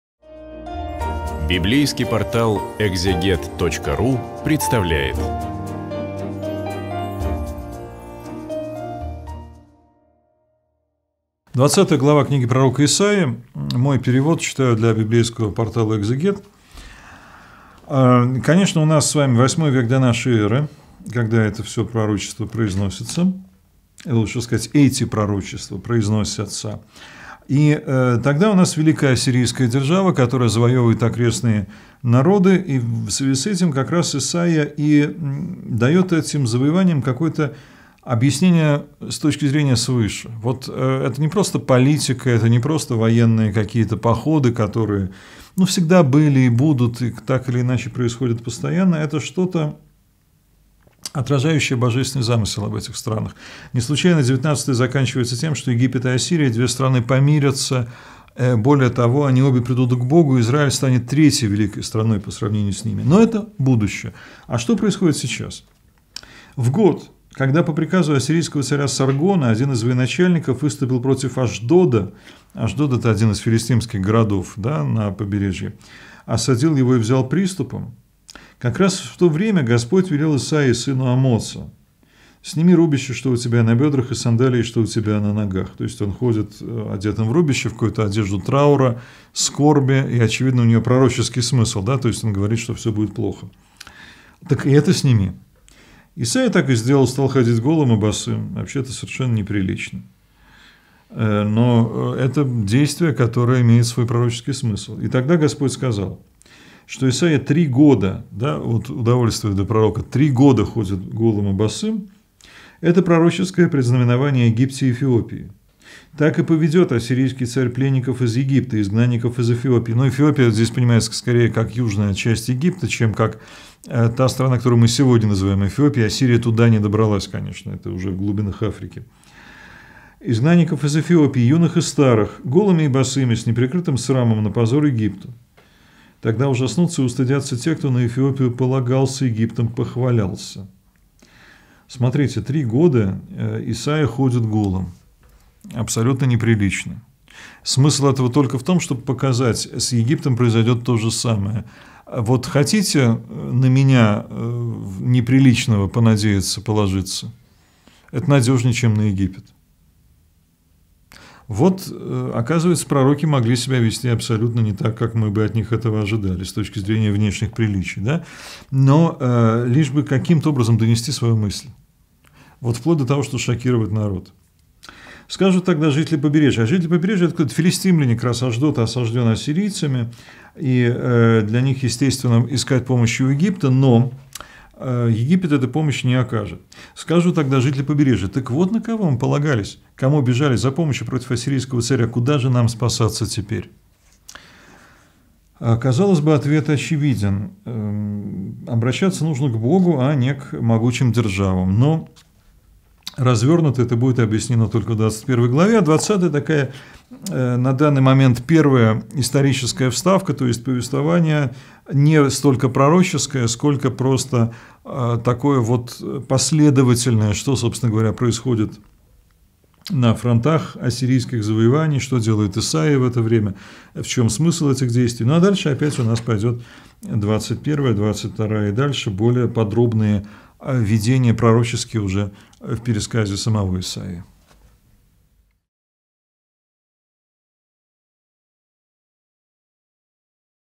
Книга пророка Исайи. Глава 20. Андрей Десницкий
В сегодняшней главе рассмотрим различные пророческие "приемы", когда Господь велит Своему избраннику сделать что-то, что может быть не понятно окружающим. Но в этих странных на первый взгляд действиях может скрываться глубокий смысл. Пробуем разобраться в этом вместе с Андреем Десницким на Экзегет.ру